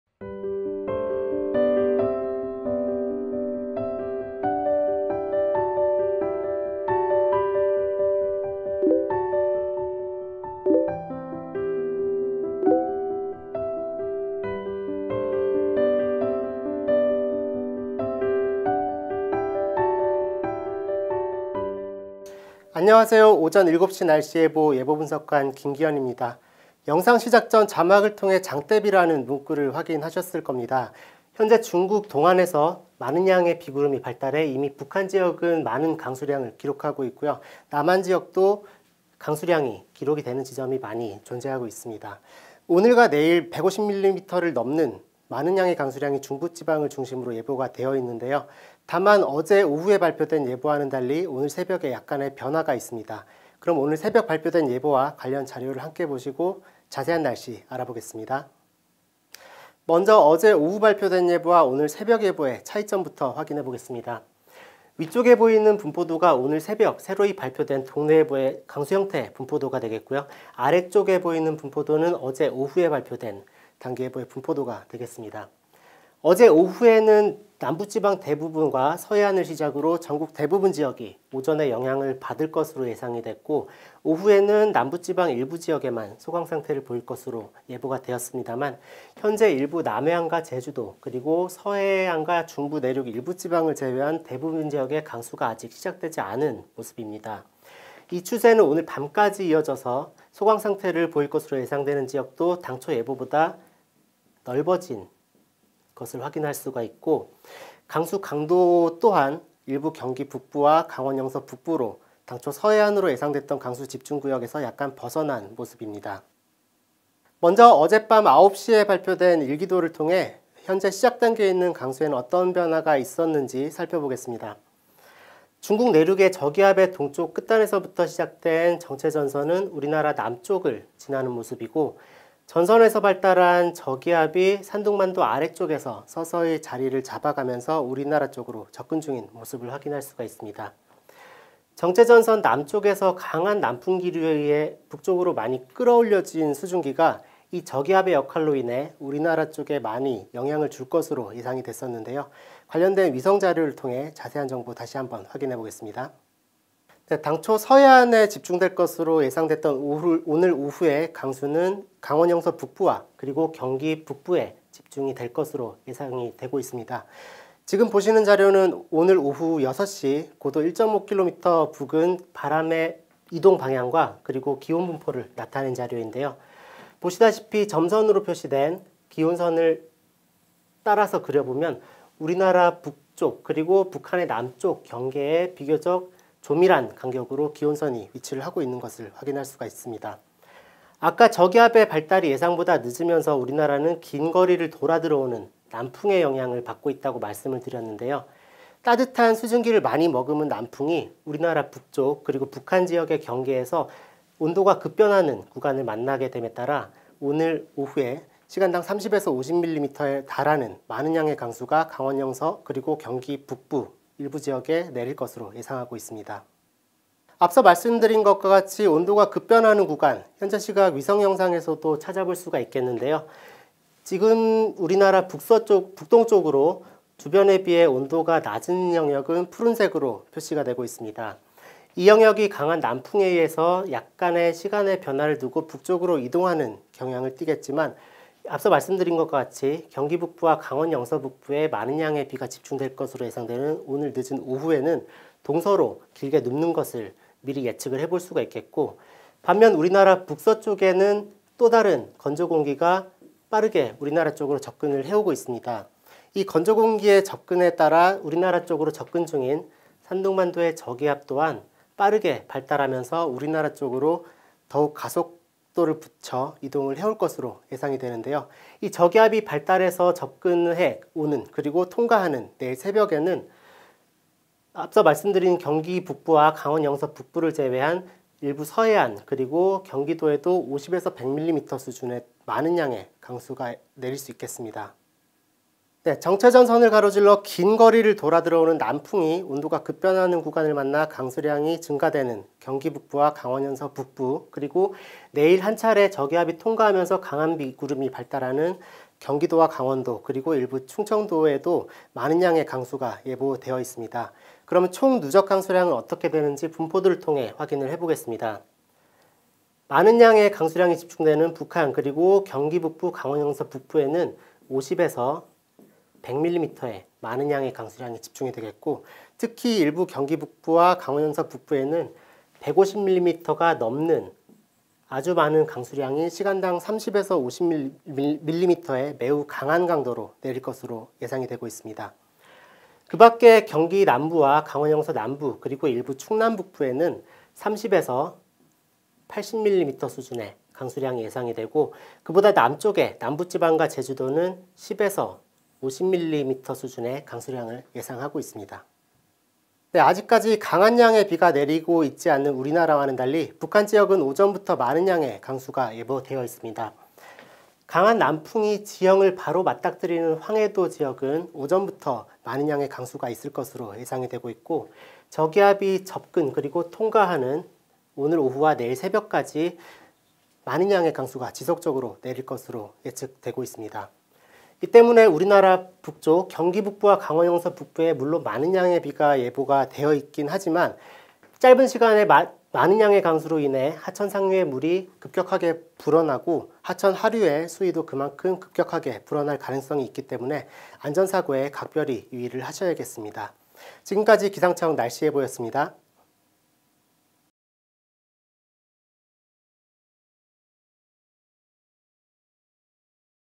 [날씨예보07] 중부지방 내일까지 장대비 내려요, 7월 19일 5시 발표
2020년 7월 19일 5시 발표된 날씨예보07입니다.

안녕하세요. 오전 7시 날씨예보 예보분석관 김기현입니다.

영상 시작 전 자막을 통해 장대비라는 문구를 확인하셨을 겁니다.
현재 중국 동안에서 많은 양의 비구름이 발달해
이미 북한 지역은 많은 강수량을 기록하고 있고요.
남한 지역도 강수량이 기록이 되는 지점이 많이 존재하고 있습니다.
오늘과 내일 150mm를 넘는 많은 양의 강수량이
중부지방을 중심으로 예보가 되어 있는데요.
다만 어제 오후에 발표된 예보와는 달리
오늘 새벽에 약간의 변화가 있습니다.
오늘 새벽 발표된 예보와 관련 자료를 함께 보시고
자세한 날씨 알아보겠습니다.

먼저 어제 오후 발표된 예보와 오늘 새벽 예보의
차이점부터 확인해 보겠습니다.
위쪽에 보이는 분포도가 오늘 새벽 새로 발표된
동네예보의 강수형태 분포도가 되겠고요.
아래쪽에 보이는 분포도는 어제 오후에 발표된
단기예보의 분포도가 되겠습니다.
어제 오후에는 남부지방 대부분과 서해안을 시작으로
전국 대부분 지역이 오전에 영향을 받을 것으로 예상이 됐고
오후에는 남부지방 일부 지역에만
소강상태를 보일 것으로 예보가 되었습니다만
현재 일부 남해안과 제주도 그리고
서해안과 중부내륙 일부 지방을 제외한 대부분 지역에
강수가 아직 시작되지 않는 모습입니다.
이 추세는 오늘 밤까지 이어져서
소강상태를 보일 것으로 예상되는 지역도
당초 예보보다 넓어진 것을 확인할 수가 있고
강수 강도 또한 일부 경기북부와 강원영서 북부로
당초 서해안으로 예상됐던 강수 집중 구역에서 약간 벗어난 모습입니다.

먼저 어젯밤 9시에 발표된 일기도를 통해
현재 시작 단계에 있는 강수에는 어떤 변화가 있었는지 살펴보겠습니다.
중국 내륙에 저기압의 동쪽 끝단에서부터 시작된
정체전선은 우리나라 남쪽을 지나는 모습이고
전선에서 발달 저기압이 산둥반도 아래쪽에서
서서히 자리를 잡아가면서
우리나라 쪽으로 접근  중인 모습을 확인할 수가 있습니다.
정체전선 남쪽에서 강한 남풍 기류에 의해
북쪽으로 많이 끌어올려진 수증기가 이 저기압의 역할로 인해
우리나라 쪽에 많이 영향을 줄 것으로 예상이 됐었는데요.
관련된 위성자료를 통해 자세한 정보 다시 한번 확인해 보겠습니다.

당초 서해안에 집중될 것으로 예상됐던 오늘 오후의 강수는
강원영서북부와 그리고 경기북부에
집중이 될 것으로 예상이 되고 있습니다.
지금 보시는 자료는 오늘 오후 6시
고도 1.5km 부근 바람에 이동방향과
그리고 기온 분포를 나타낸 자료인데요.
보시다시피 점선으로 표시된 기온선을 따라서 그려보면
우리나라 북쪽 그리고 북한의 남쪽 경계에 비교적 조밀한 간격으로
기온선이 위치를 하고 있는 것을 확인할 수가 있습니다.
아까 저기압에 발달이 예상보다는 늦으면서
우리나라는 긴 거리를 돌아 들어오는
남풍의 영향을 받고 있다고 말씀을 드렸는데요.
따뜻한 수증기를 많이 머금은 남풍이 우리나라 북쪽
그리고 북한 지역의 경계에서 온도가 급변하는 구간을 만나게 됨에 따라
오늘 오후에 시간당 30~50mm에 달하는 많은 양의 강수가
강원영서 그리고 경기북부 일부 지역에 내릴 것으로 예상하고 있습니다.

앞서 말씀드린 것과 같이 온도가 급변하는 구간을
현재 시간 위성영상에서도 찾아볼 수가 있겠는데요.
지금 우리나라 북동쪽으로 주변에 비해 온도가 낮은 영역은
푸른색으로 표시가 되고 있습니다.
이 영역이 강한 남풍에 의해서 약간의 시간에 변화를 두고
북쪽으로 이동하는 경향을 띄겠지만
앞서 말씀드린 것과 같이 경기북부와 강원영서 북부에
많은 양의 비가 집중될 것으로 예상되는 오늘 늦은 오후에는
동서로 길게 눕는 것을 미리 예측을 해볼 수가 있겠고
반면 우리나라 북서쪽에는 또 다른 건조 공기가
빠르게 우리나라 쪽으로 접근을 해 오고 있습니다.
이 건조공기의 접근에 따라
우리나라 쪽으로 접근 중인 산둥반도에 저기압 또한
빠르게 발달하면서 우리나라 쪽으로
더욱 가속도를 붙여 이동을 해올 것으로 예상이 되는데요.
이 저기압이 발달해서 접근해오는 그리고 통과하는 내일 새벽에는
앞서 말씀드린 경기북부와 강원영서북부를 제외한
일부 서해안 그리고 경기도에도 50~100mm 수준의
많은 양의 강수가 내릴 수 있겠습니다.

정체전선을 가로질러 긴 거리를 돌아 들어오는 남풍이
온도가 급변하는 구간을 만나 강수량이 증가되는
경기북부와 강원영서북부
그리고 내일 한차례 저기압이 통과하면서
강한 비구름이 발달하는 경기도와 강원도
그리고 일부 충청도에도 많은 양의 강수가 예보가 되어있습니다.
그러면 총 누적 강수량은 어떻게 되는지
분포도를 통해 확인을 해 보겠습니다.
많은 양의 강수량이 집중되는 북한
그리고 경기북부, 강원영서북부에는
50~100mm의 많은 양의 강수량이 집중되겠고
특히 일부 경기북부와 강원영서 북부에는 150mm가 넘는
아주 많은 강수량이 시간당 30~50mm의
매우 강한 강도로 내릴 것으로 예상되고 있습니다.
그밖에 경기남부와 강원영서 남부
그리고 일부 충남 북부에는 30~80mm 수준의 강수량이 예상이 되고
그 보다 남쪽에 남부지방과 제주도는
10~50mm 수준의 강수량을 예상하고 있습니다.

아직까지 강한 양의 비가 내리고 있지 않는 우리나라와는 달리
북한 지역은 오전부터 많은 양의 강수가 예보되어 있습니다.
강한 남풍이 지형을 바로 맞닥뜨리는 황해도 지역은
오전부터 많은 양의 강수가 있을 것으로 예상이 되고 있고
저기압이 접근 그리고 통과하는 오늘 오후와 내일 새벽까지
많은 양의 강수가 지속적으로 내릴 것으로 예측되고 있습니다.
이 때문에 우리나라 북쪽 경기북부와 강원영서북부에
물론 많은 양의 비가 예보가 되어 있기는 하지만
짧은 시간에 많은 양의 강수로 인해
하천 상류에 물이 급격하게 불어나고
하천 하류에 수위도 그만큼 급격하게 불어날 가능성이 있기 때문에
안전사고에 각별히 유의를 하셔야겠습니다.

지금까지 기상청 날씨예보였습니다.